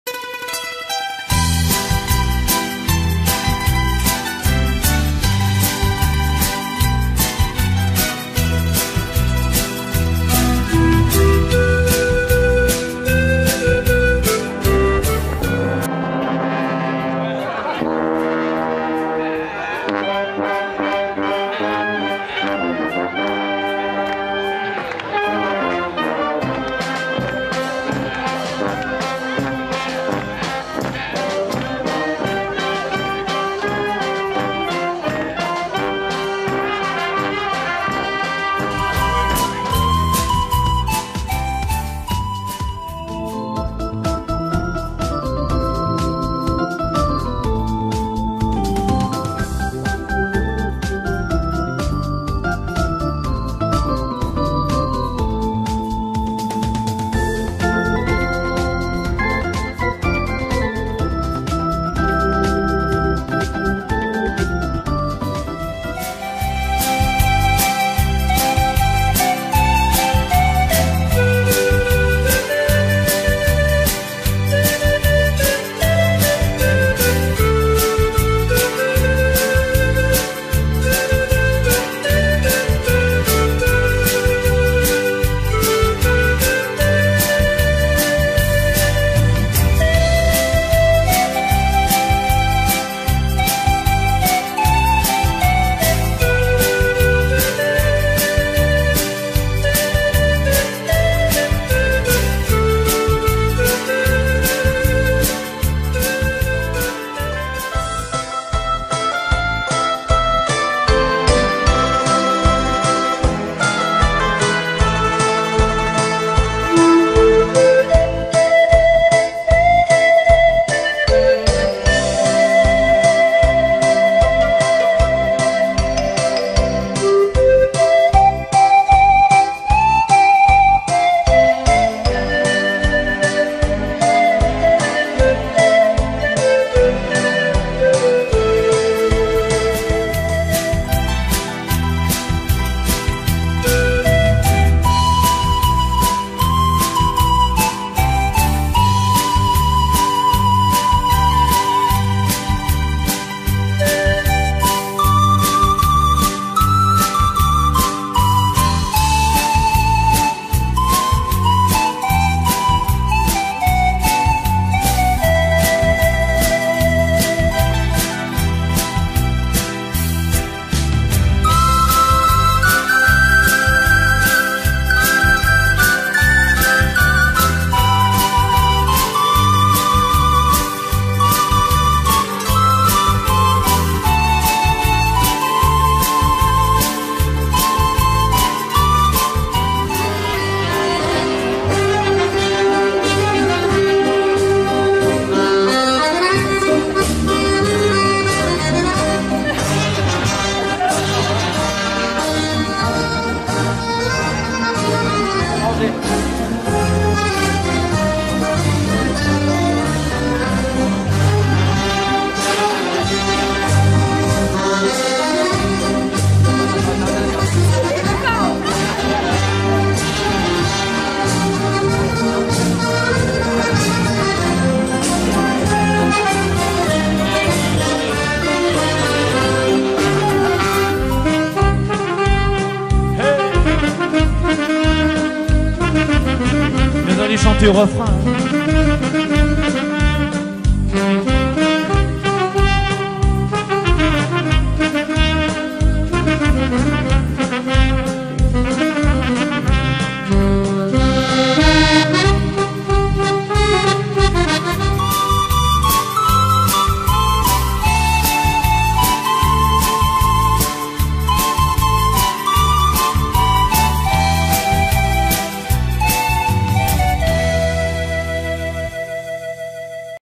A la foire aux chèvres, de St Aupre.
Ancestrale et authentique, la foire aux biquettes de St Aupre
est devenue la grande fête populaire de la fin d'été.
Un grand moment convivial autour du tilleul séculaire.